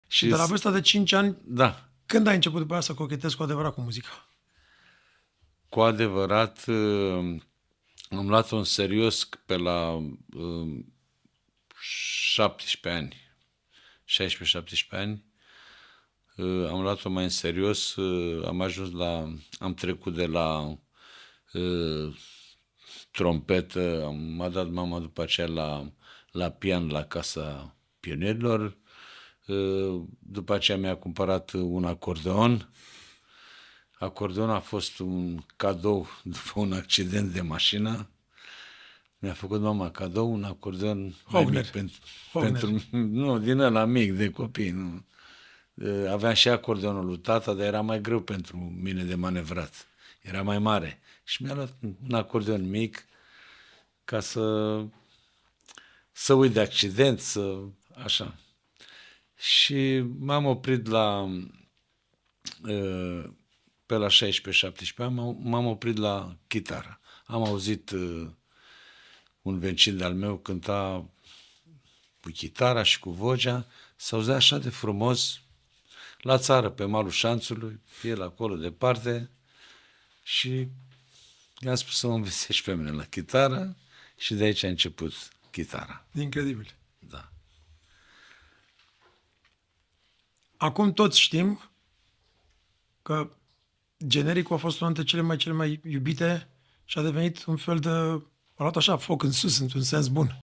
Destinul meu muzical: Povestea mea de la acordeon la chitară
Ascultă cu atenție discuțiile captivante și inspiraționale din podcastul lui  @DamianDraghici.   Explorează conversațiile lor de profunzime, împărtășind perspective valoroase și insight-uri unice.